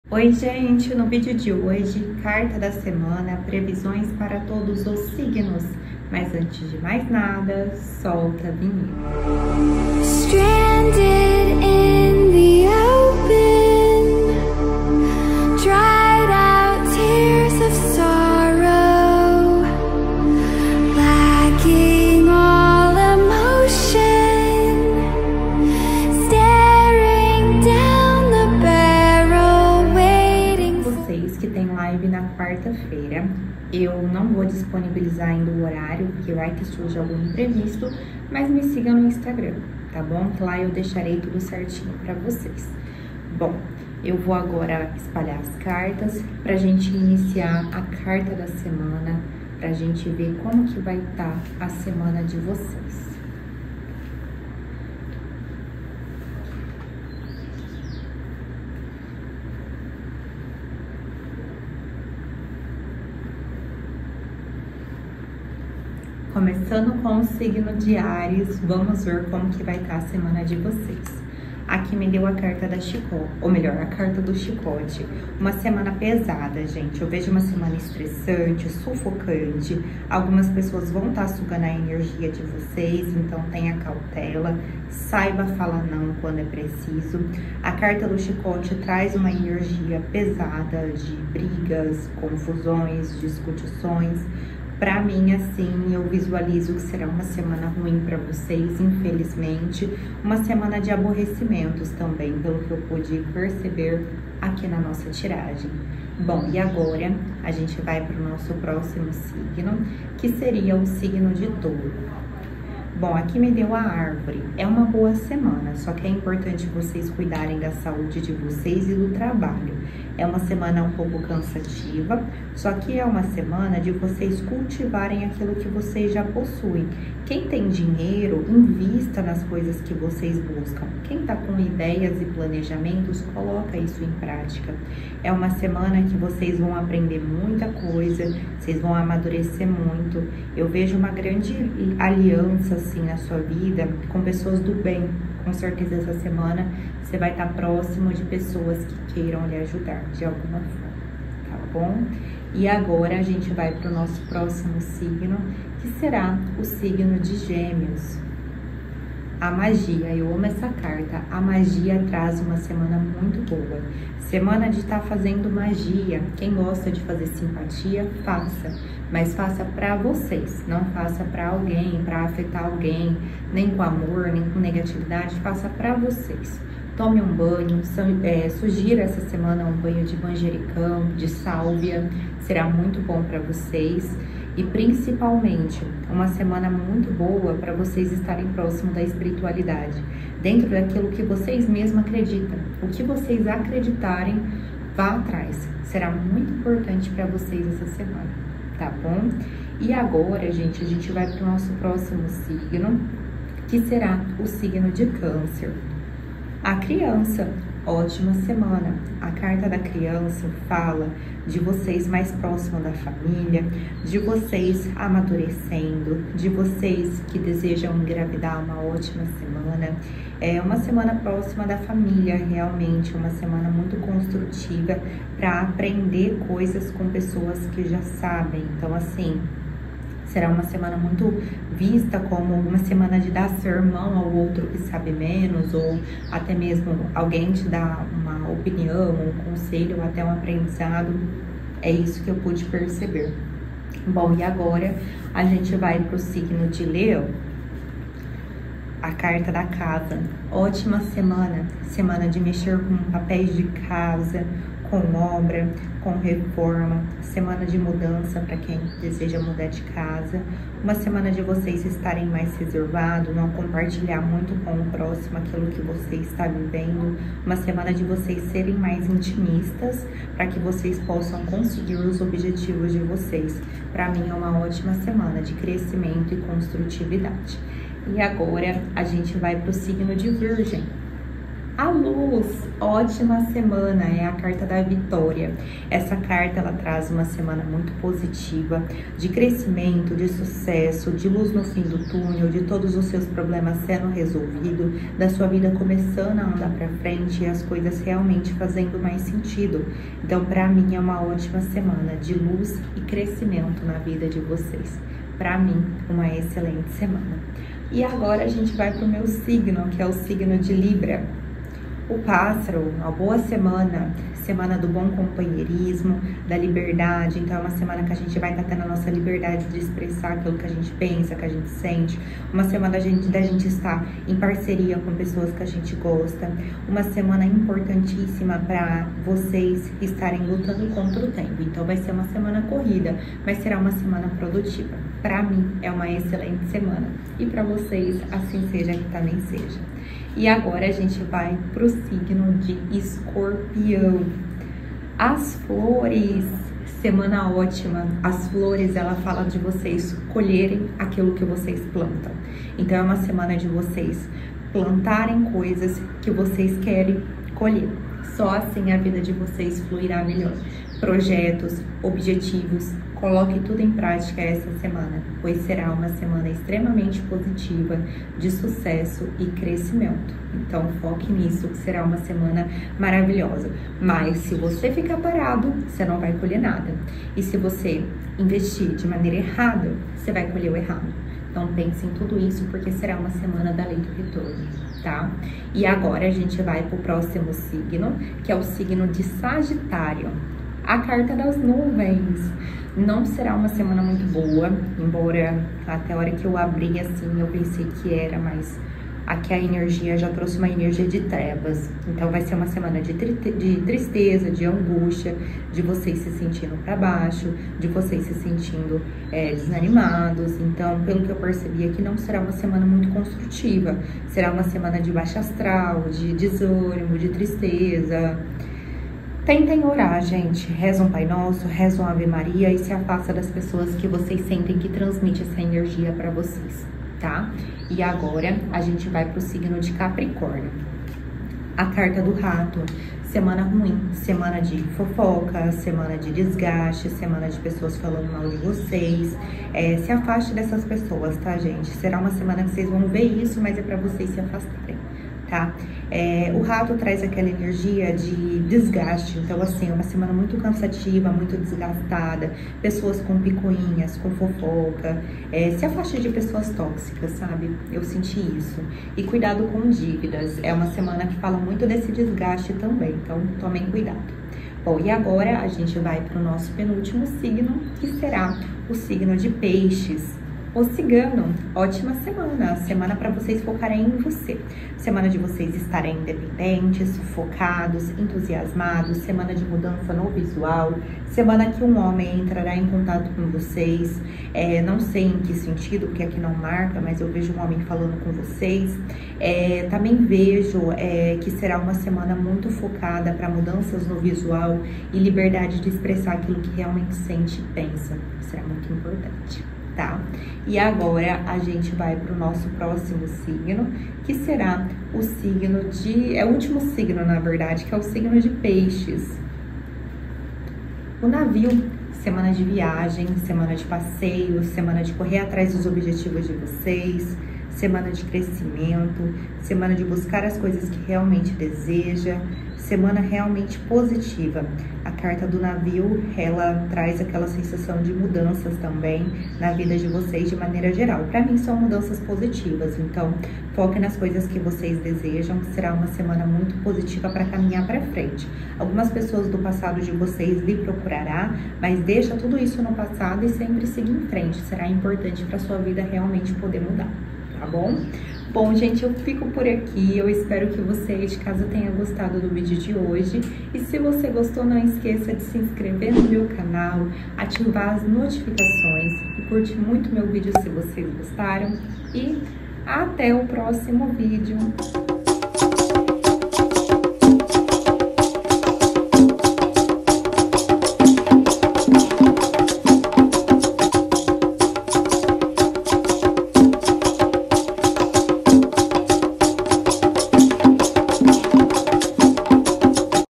HORÓSCOPO 22/1 até dia 28/1 CARTA DA SEMANA PREVISÕES PARA TODOS OS SIGNOS
Horóscopo carta da semana previsões para todos os signos
Mês de janeiro